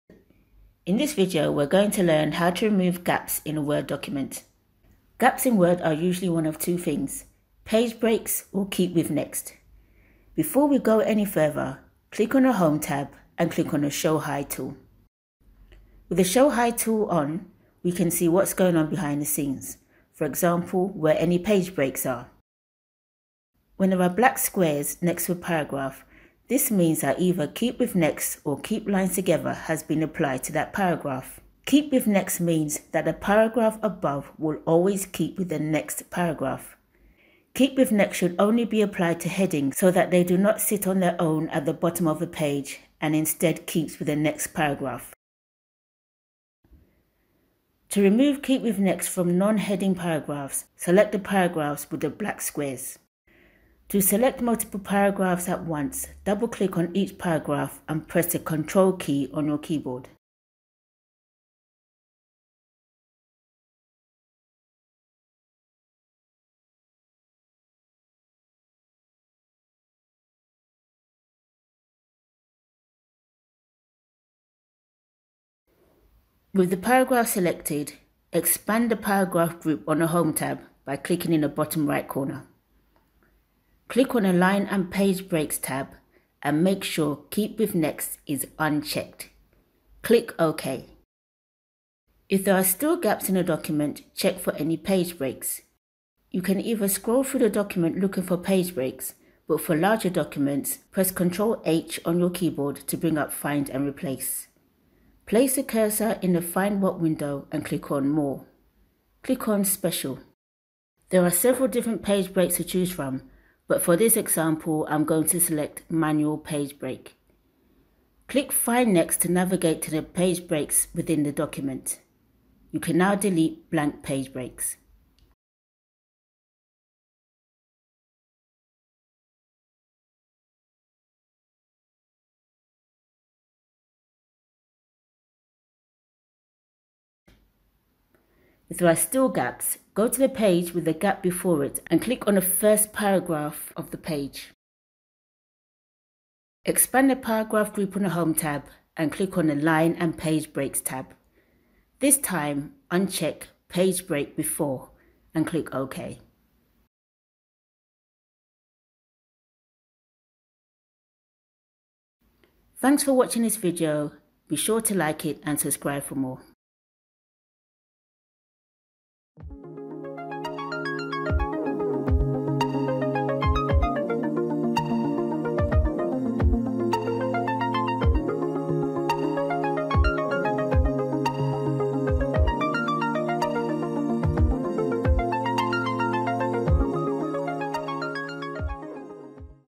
How to close large paragraph gaps in Word
How to close large gaps in Word

Learn how to close large gaps in a Word document - paragraphs starting on new page